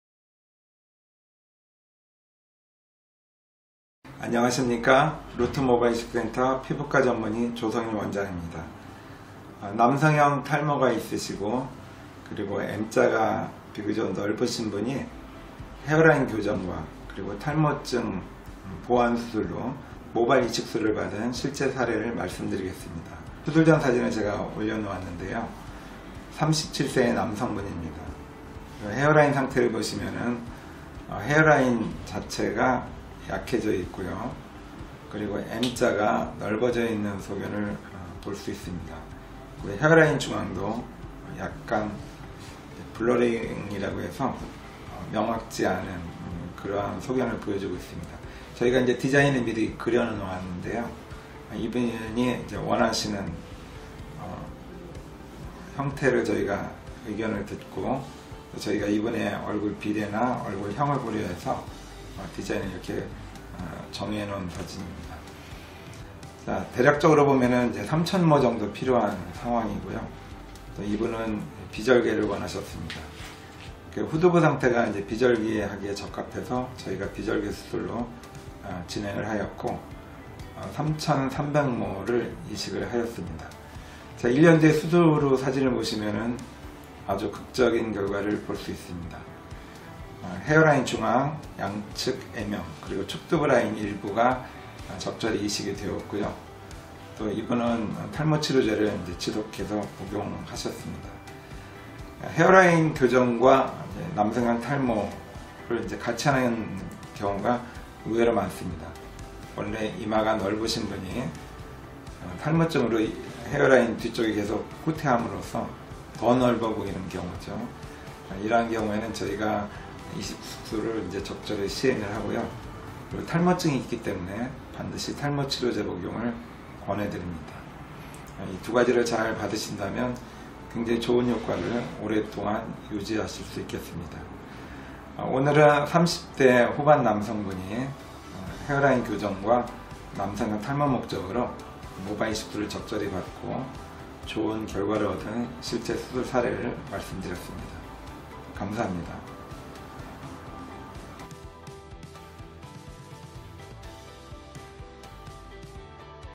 모발이식 M자탈모 헤어라인 동시교정 모발이식 비절개 3300모이식 전 후
남성형탈모와 헤어라인 교정을 하루에 해결합니다.